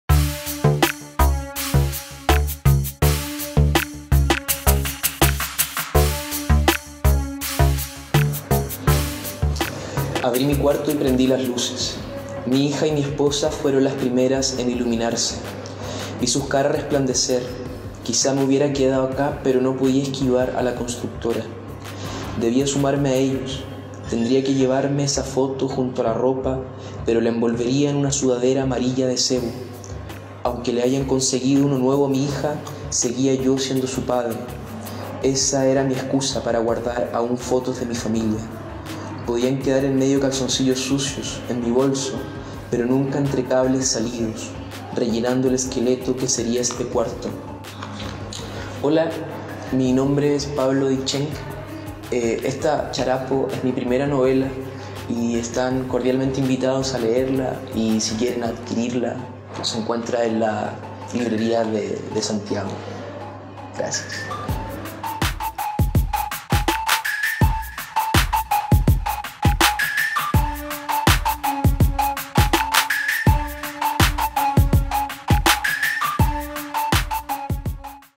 CHARAPO (extracto) por Pablo D. Sheng
Conversamos con Pablo D. Sheng, el joven autor de la novela de ficción "CHARAPO" de tremenda empatía que nos coloca en el mundo de un inmigrante peruano en Chile.

"Pablo D. Sheng nació en 1995. Recoletano, fanático de Colo-Colo y del cine oriental. Tiene fobia a las palomas y cree que escribiendo de ellas puede superarlo. Familiarizado con el barrio de Patronato, que recorre cotidianamente como el protagonista de su novela. Forma parte del colectivo de poesía Neotaku Monkey D. Luffy, que ya ha organizado dos lecturas públicas en el bar Bella China. Su primera novela, Charapo, empieza cuando un padre de familia se despide de su esposa e hija para probar suerte en Santiago." - COLERA.CL

"‪#‎Charapo‬ es un aventura entrañable y al mismo tiempo una historia impetuosa ,relatada con la intensidad de un bolero" - La Horca

"Charapo confirma la vitalidad de la narrativa chilena joven, con nuevos nombres y autores, cuya lectura es harto más atractiva y provocadora que la de los ancianos de la tribu..." - El Mercurio